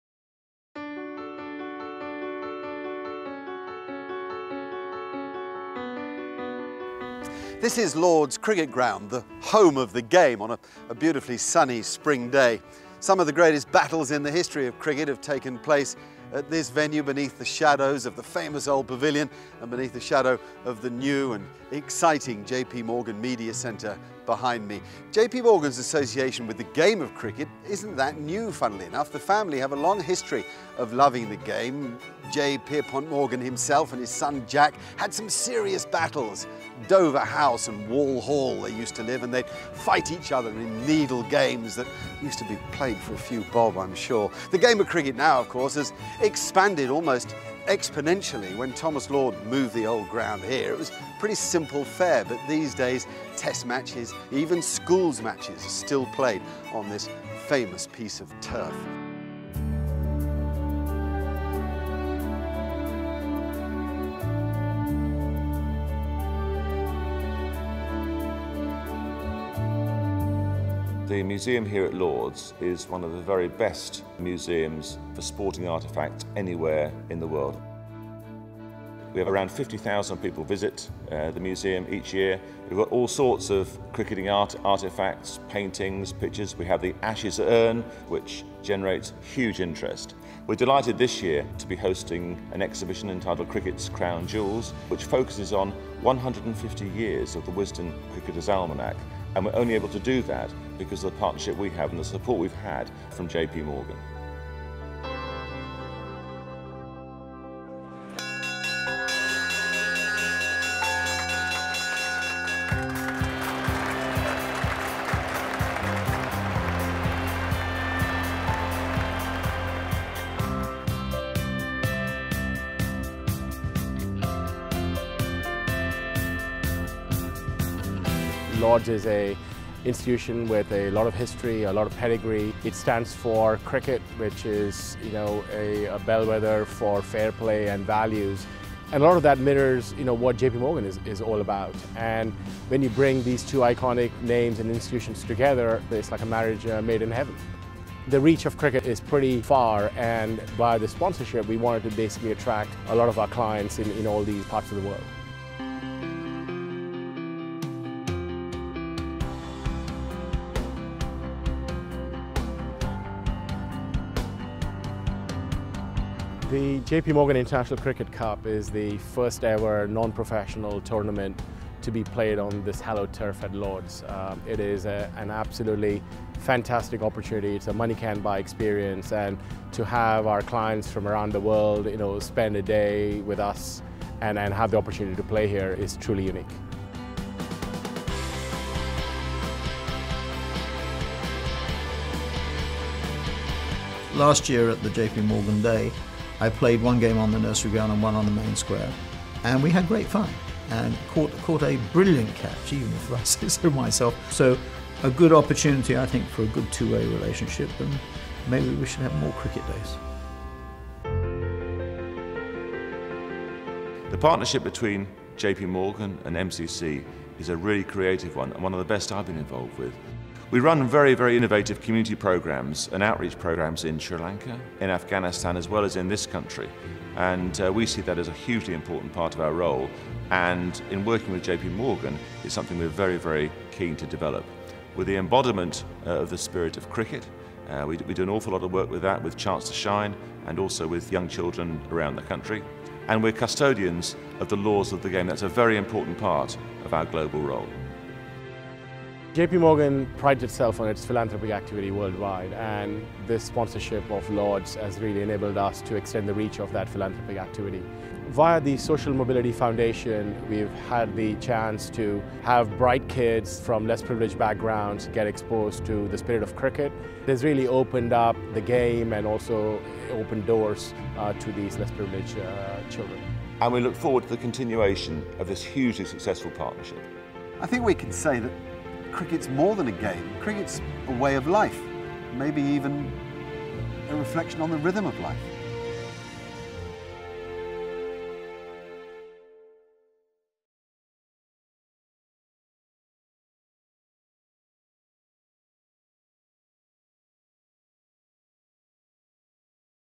Mark Nicholas - More than a game | Everything you ever wanted to know about
Mark Nicholas revisits the early stages of Cricket as a sport, the founding of Lord's Cricket ground and the close history of J.P Morgan with the game of cricket in light of the new sponsorship.

This great insight into the conception of the sport its an essential watch for any Cricket historians or enthusiasts looking to gain some knowledge that you might not already know.